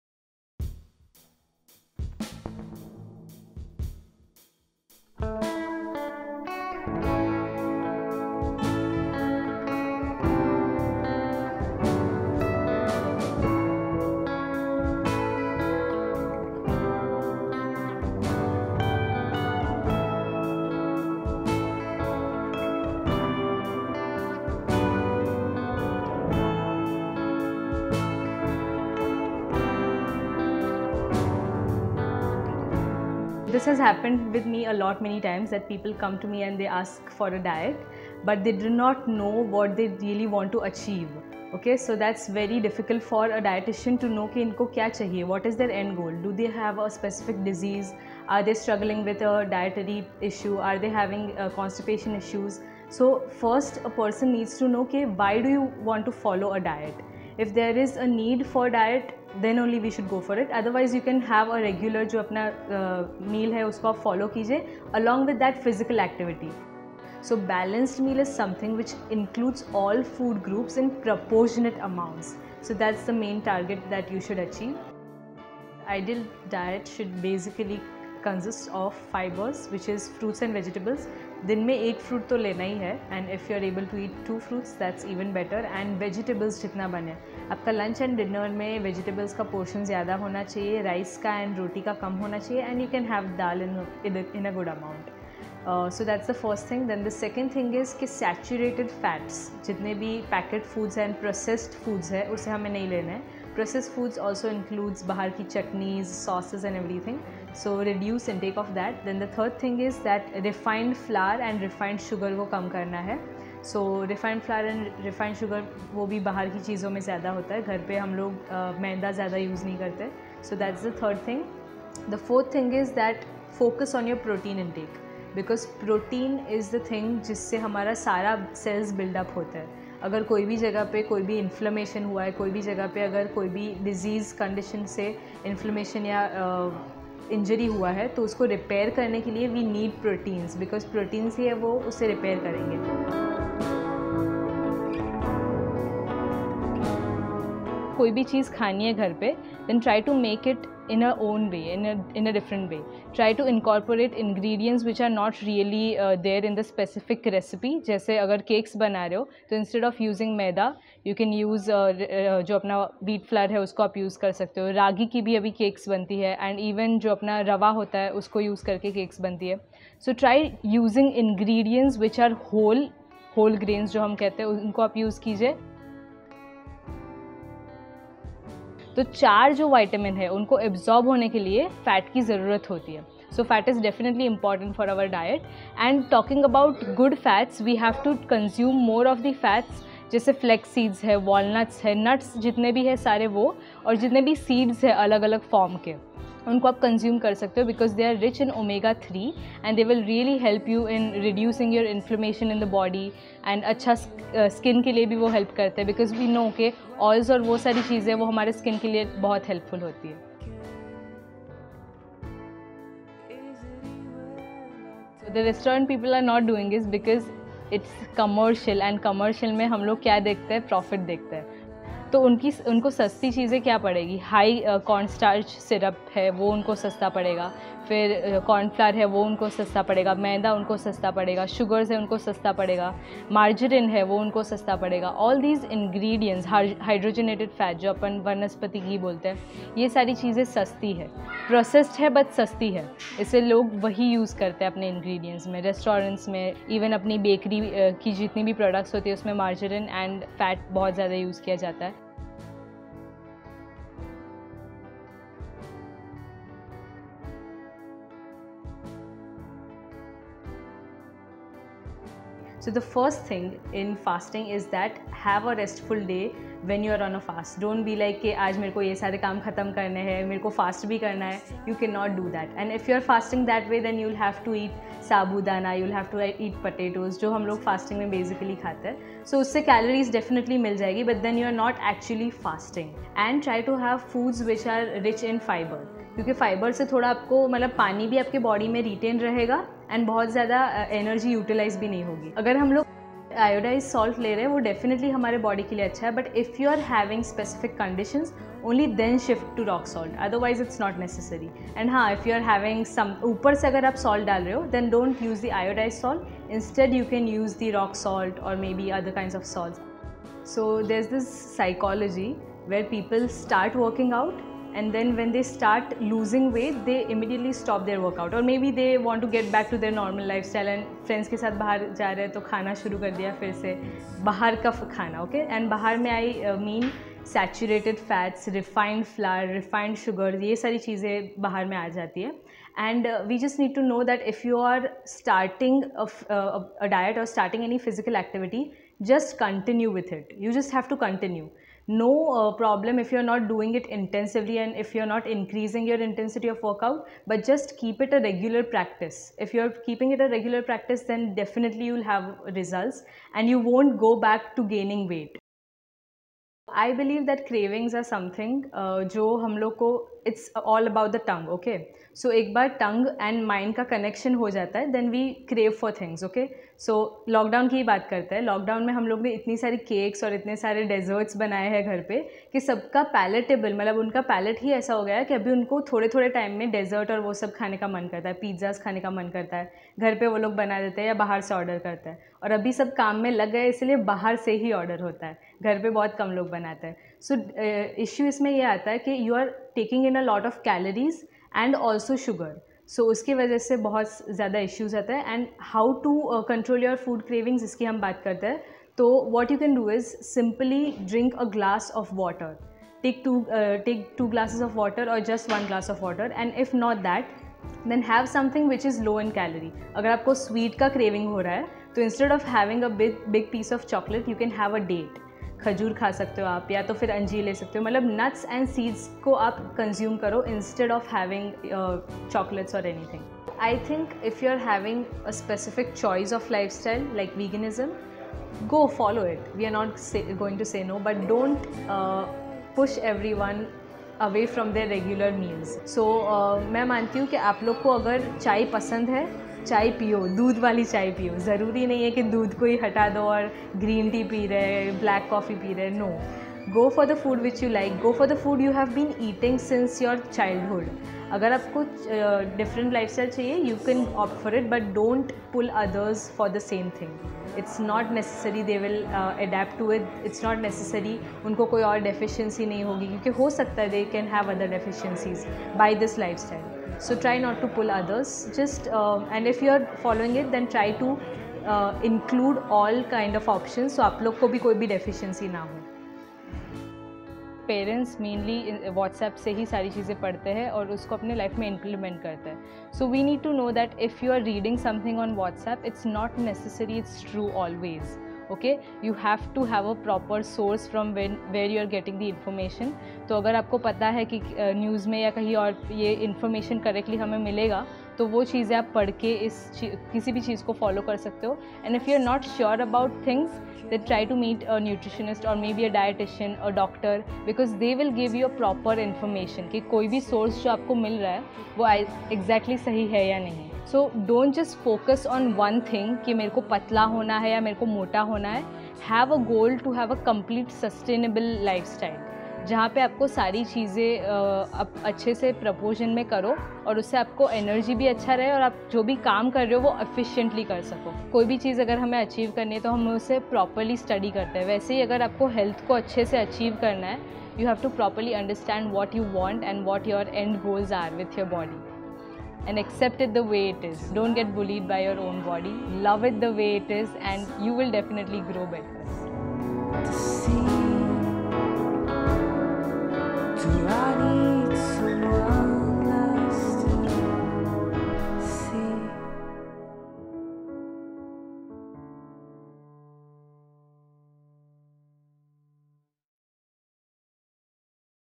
Episode 3 - Eat Smart, No Parallel Universe
featuring Aastha, A nutritionist/Dietitian from Vadodara, debunks a few myths and gives us some very vital information that everyone is supposed to know but do not. watch the 3rd episode, which is definitely going to help you make better choices when it comes to food and living a healthier lifestyle changing some of your old eating habits.